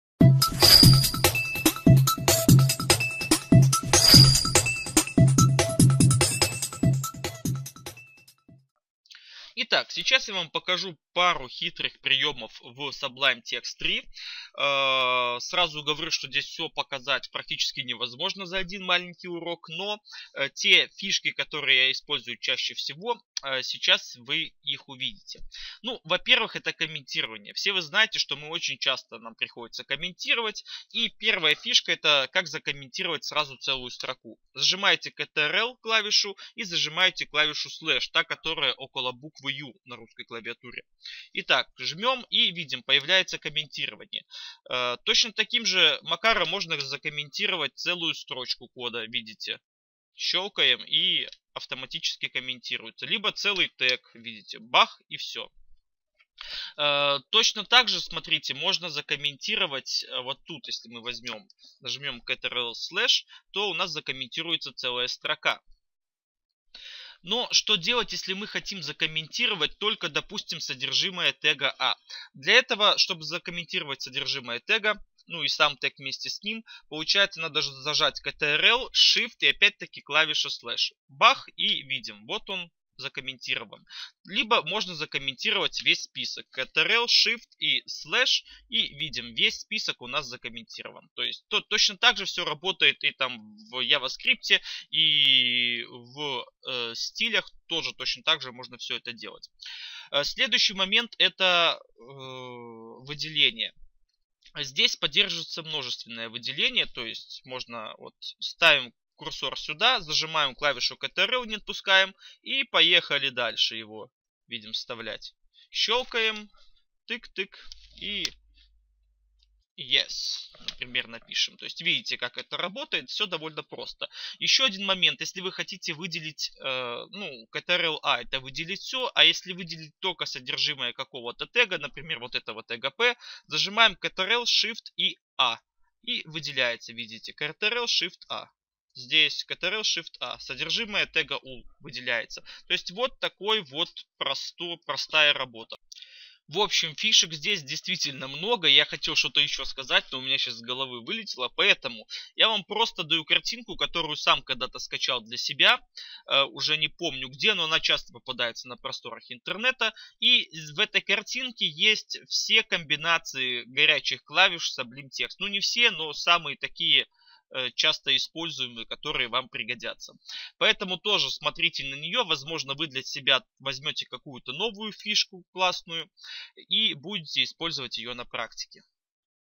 Урок 15. Маленькие секреты Sublime Text 3
(там же находятся файлы для работы)

Как быстро сделать комментарий строки?
Как быстро сделать комментарий содержимого тега?
Как выделить быстро содержимое тега? (включая и дочерние теги)
Как править код сразу в нескольких местах?

Это все - стандартный функционал редактора Sublime Text 3. И то, что я покажу в уроке - лишь капля в море. Поэтому к видеокурсу прилагается скриншот горячих клавиш Sublime Text.

Пользуйтесь на здоровье!